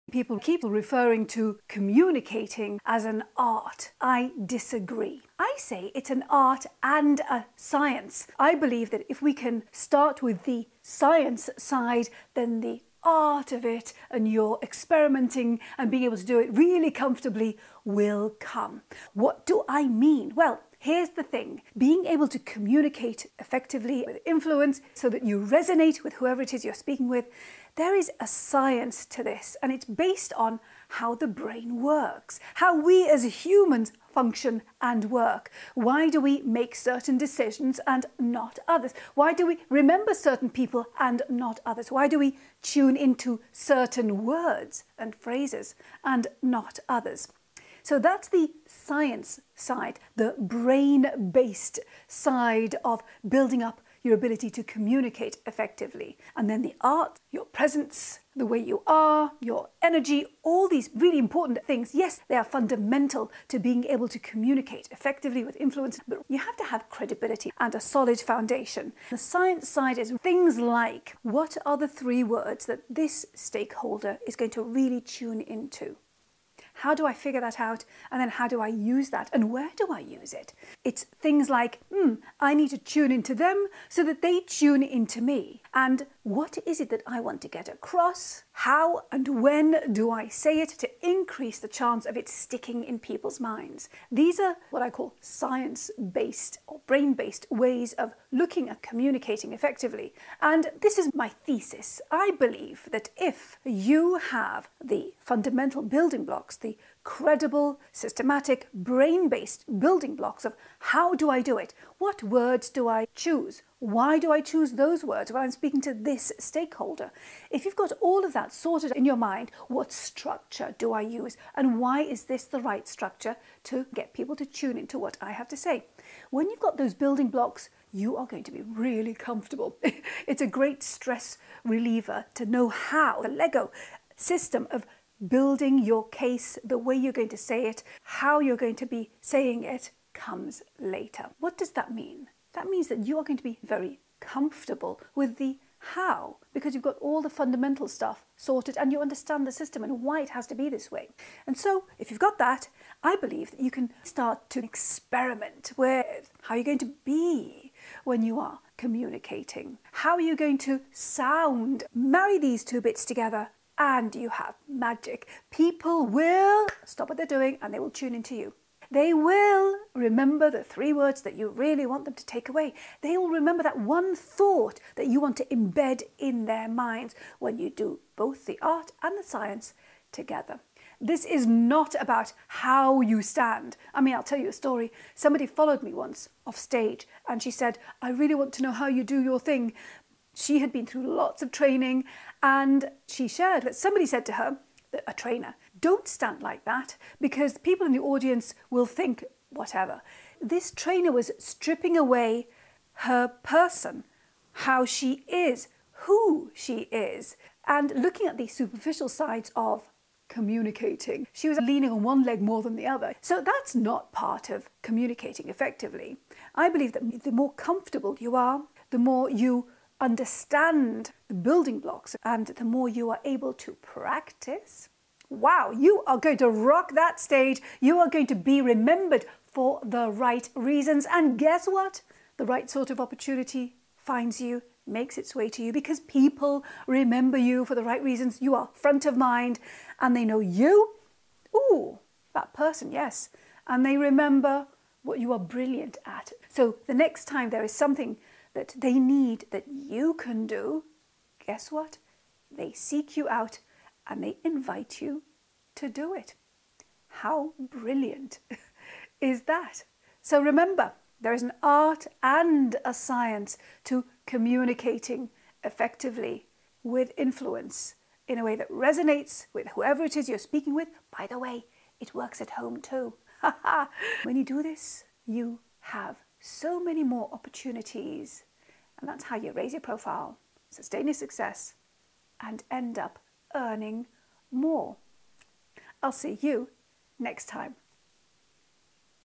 Is Communicating with Impact an Art or Science?
I share my thoughts around what you need to communicate better.

In this video you will learn:

✅ what makes a great communicator

✅ what the science & art include

✅ why it's important you do this

📣 Be The BRILLIANT Communicator!